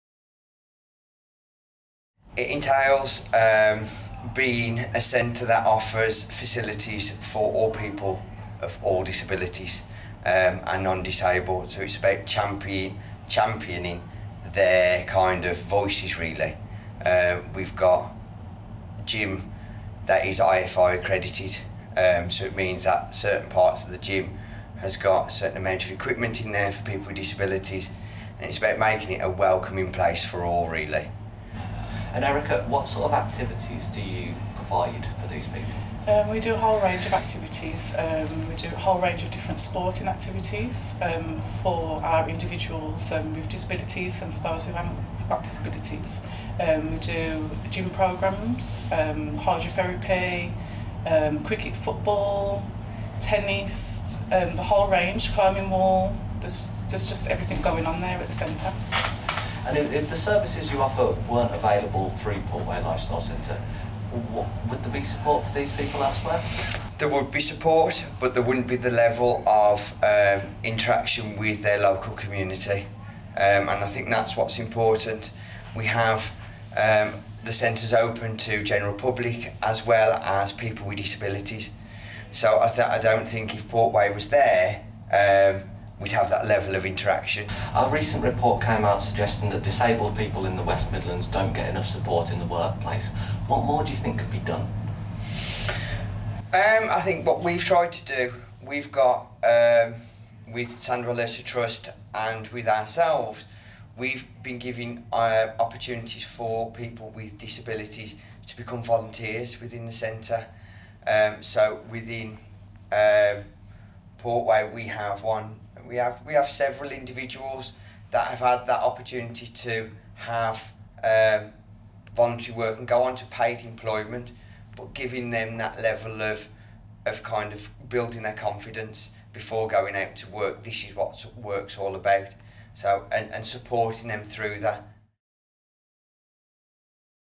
Big News - Portway Lifestyle Centre
24th June 2016. It's National Learning Disability Week and we 've been hearing from two people who work in that field, they're from the Portway Lifestyle Centre in Sandwell.

Two of their team spoke with Tom Bowen who began by asking what it took to be a centre of excellence.